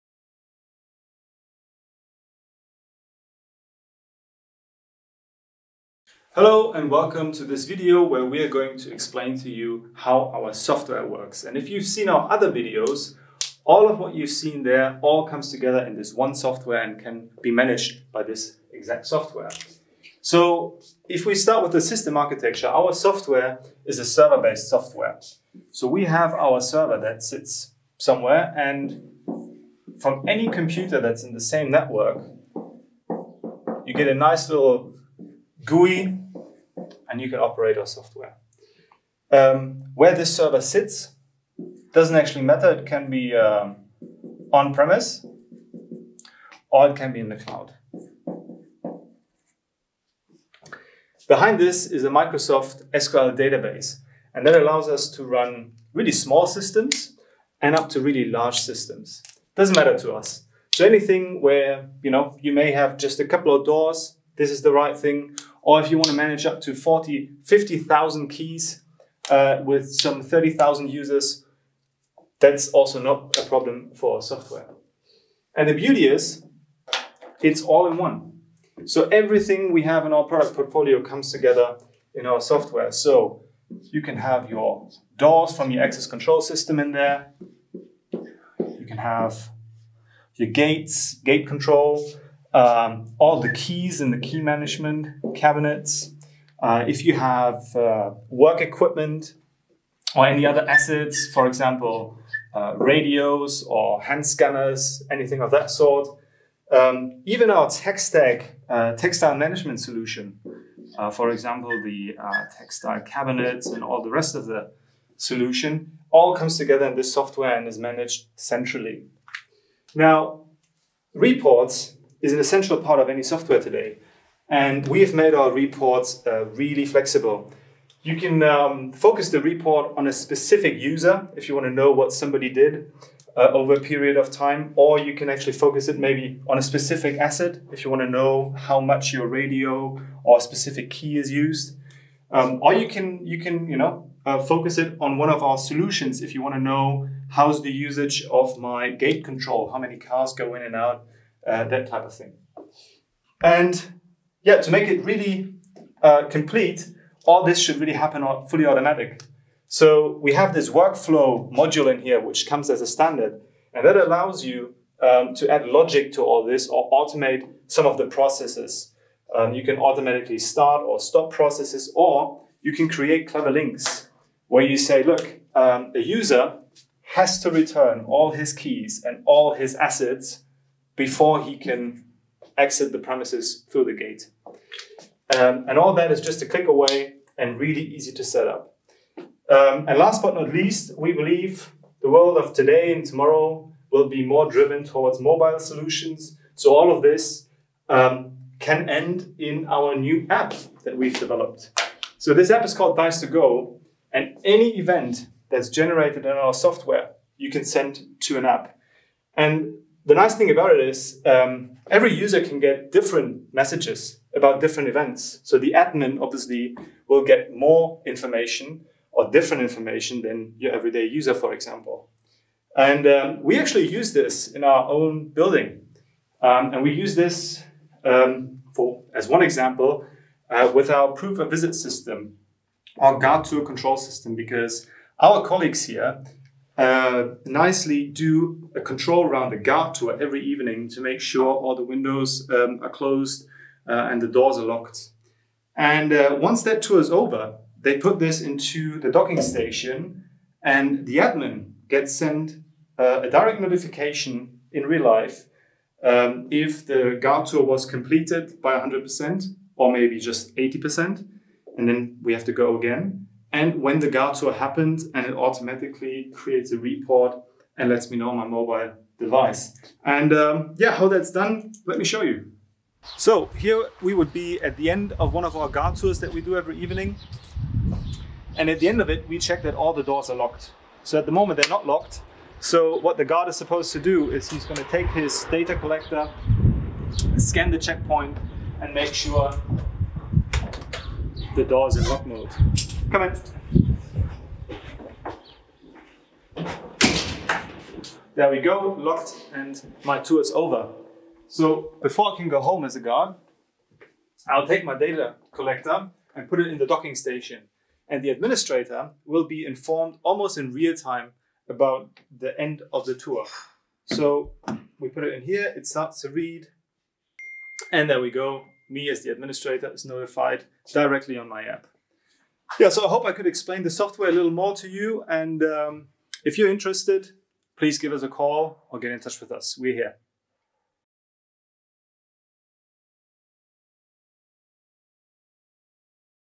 Commander Connect - The central management software for all deister electronic solutions
All products from the deister electronic product and solution portfolio can be centrally managed and intelligently linked with the browser-based and intuitive Commander Connect software.

Daily administration, monitoring and management of all systems can now be handled efficiently and in a time-saving manner.

Commander Connect can be perfectly adapted to your needs, suitable for your overall system, with various software modules available separately and can be expanded at any time.

Freely configurable reports generate individual evaluations and alarm notifications to optimally meet the various requirements - just as you need it. Data backup options, a detailed assignment of rights and the recording of software activities give you the certainty of having all processes in your system under control. At all times.

Would you like to know more about our Commander Connect software?